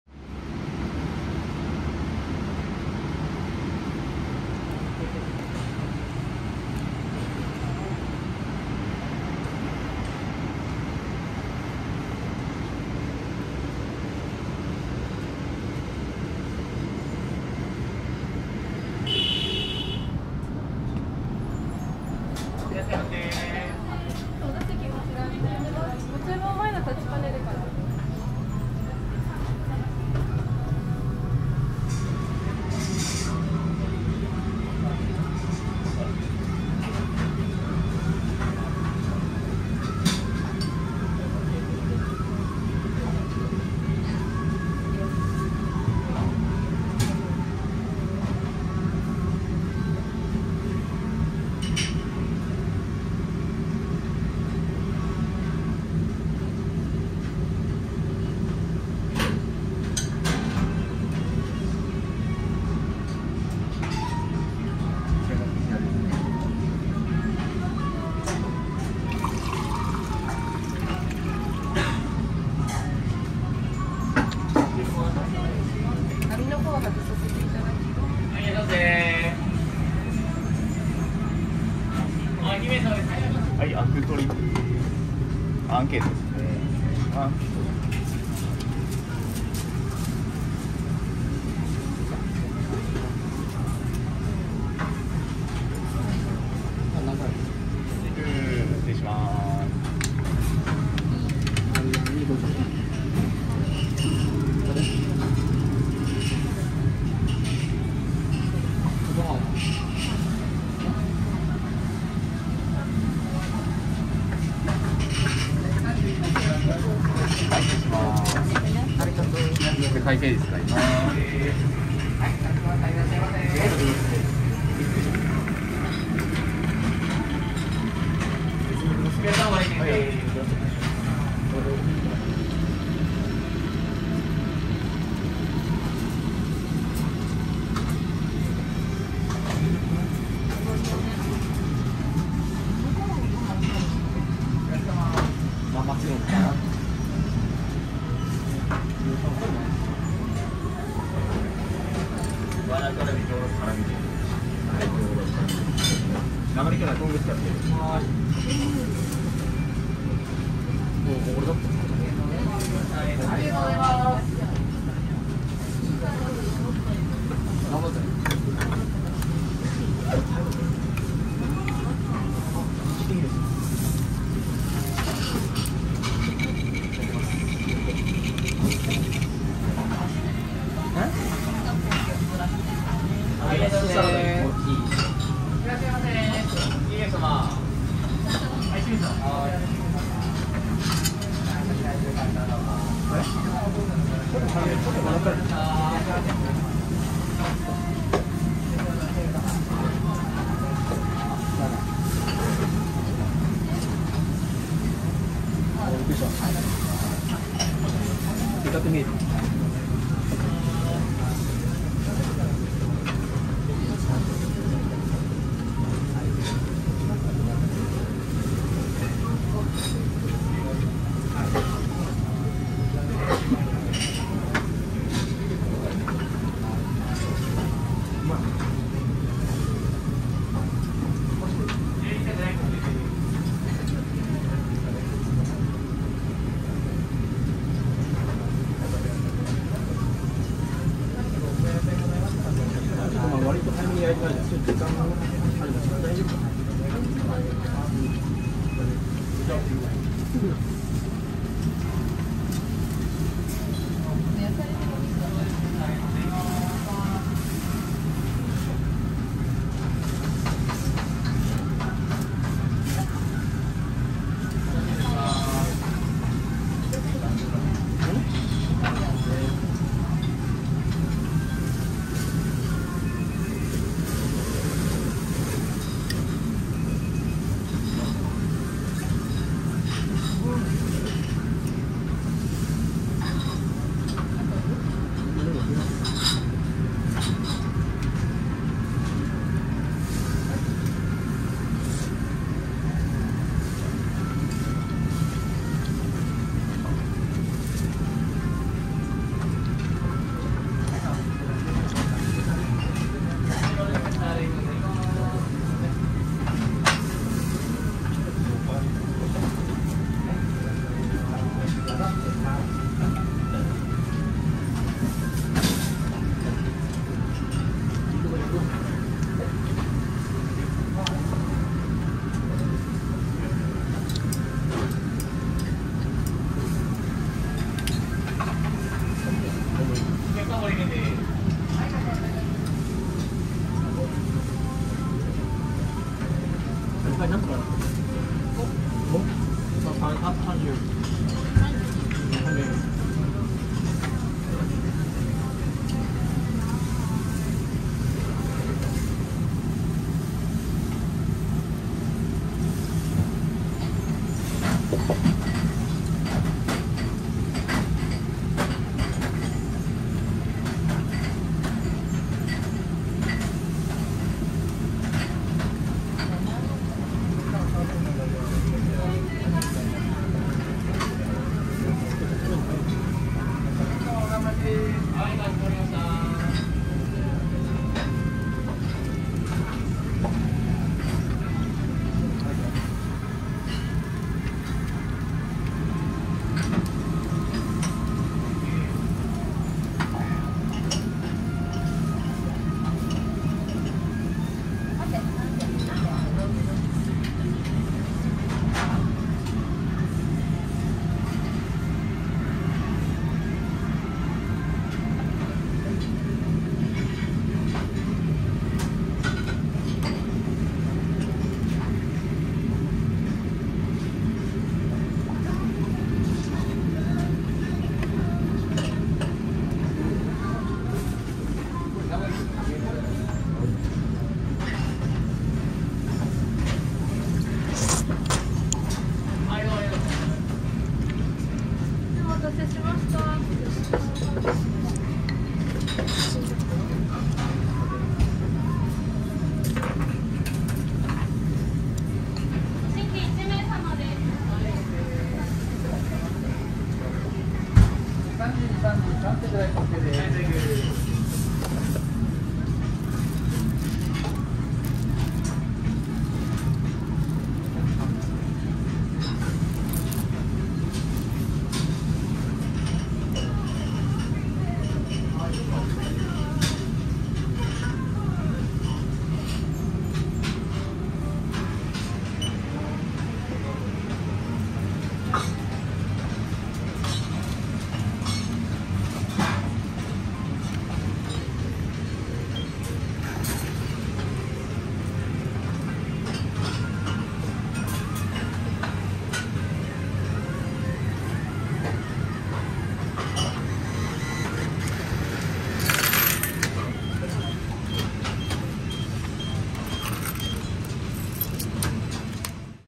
Individual Japanese Hotpot Dining Booth
Trying the limited Japanese Hotpot at Yakiniku Like